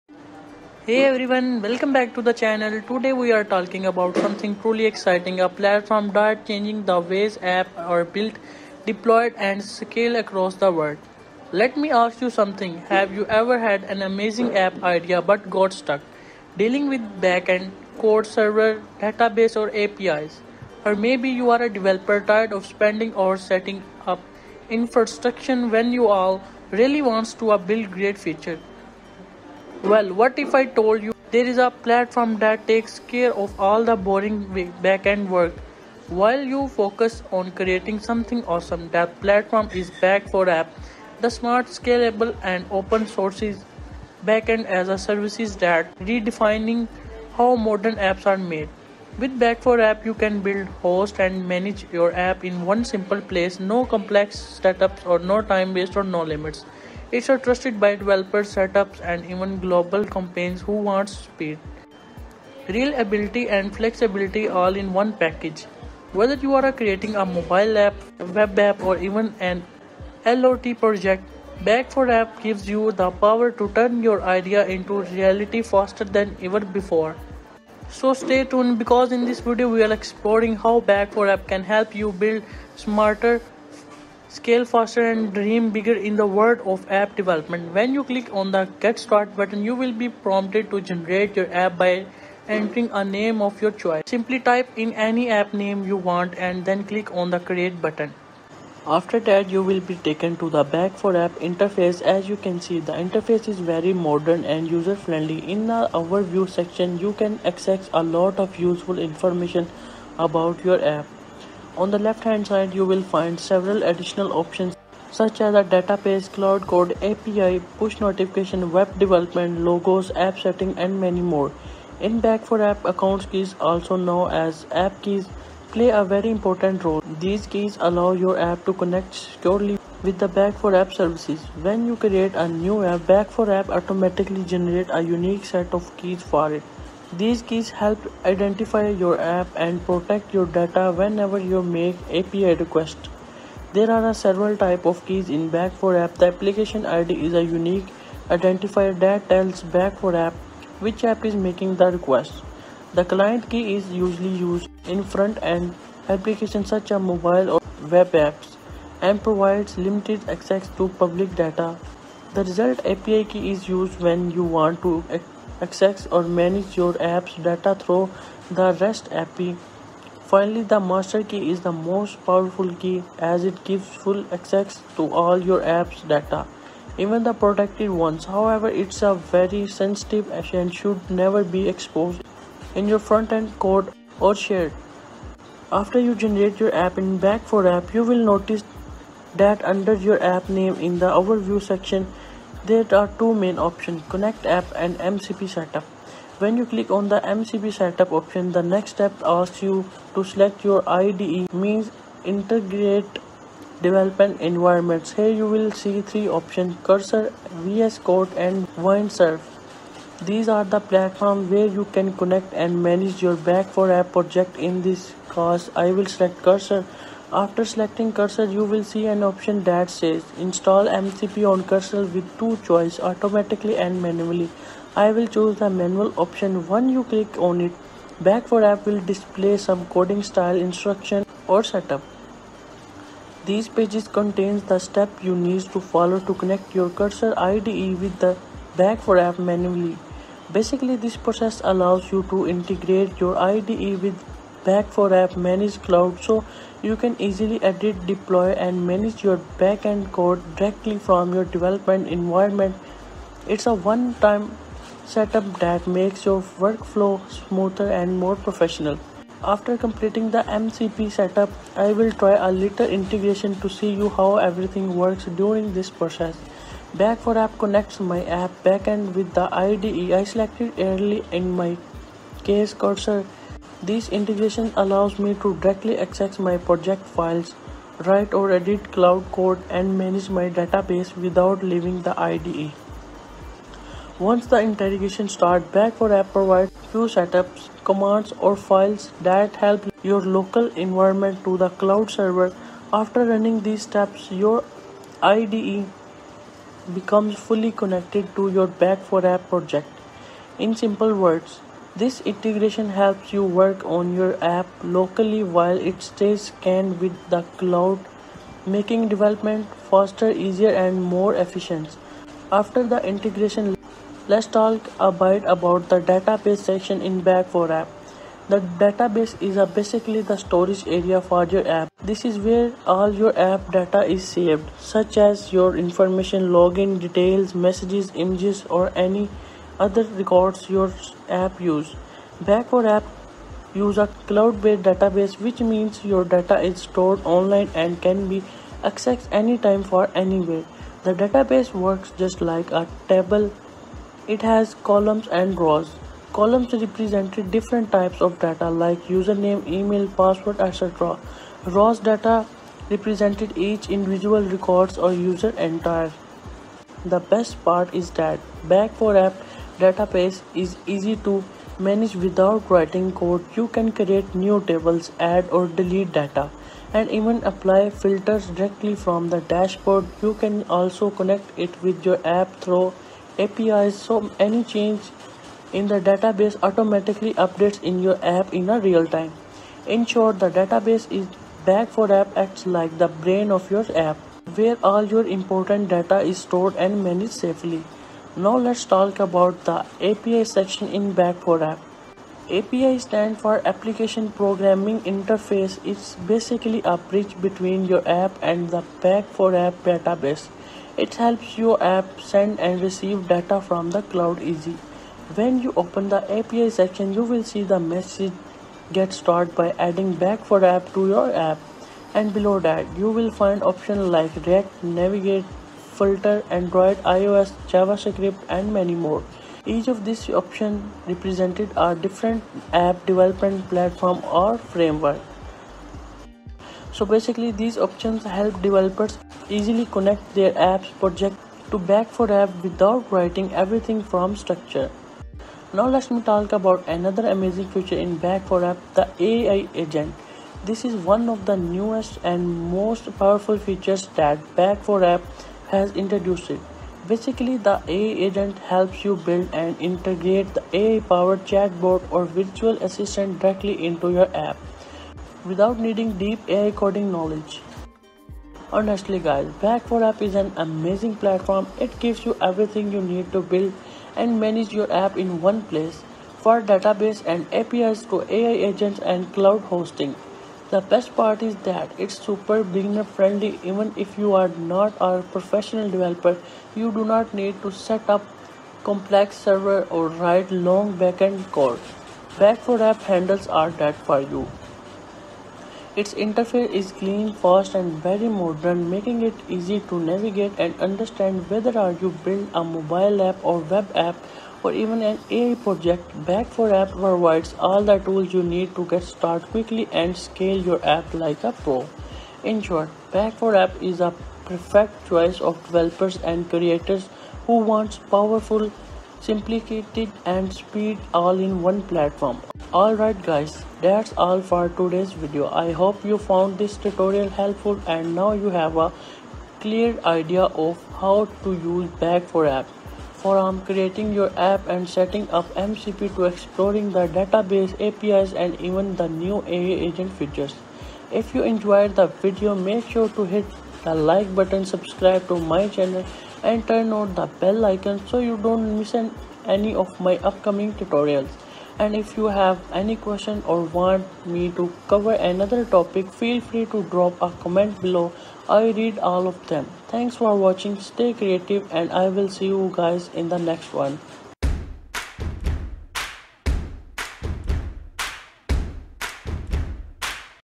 Build Powerful Apps Without Coding — Back4App Full Tutorial
In this video, I’ll show you how to build a complete backend for your app using Back4App — a low-code platform that makes app development faster and easier.
No need to manage servers, databases, or APIs manually — Back4App does it all for you!

👉 Learn how to:

Set up your project in minutes

Manage your data visually

Use REST & GraphQL APIs

Deploy your backend to the cloud

Whether you’re a beginner or an experienced developer, this tool will save you time and help you scale faster.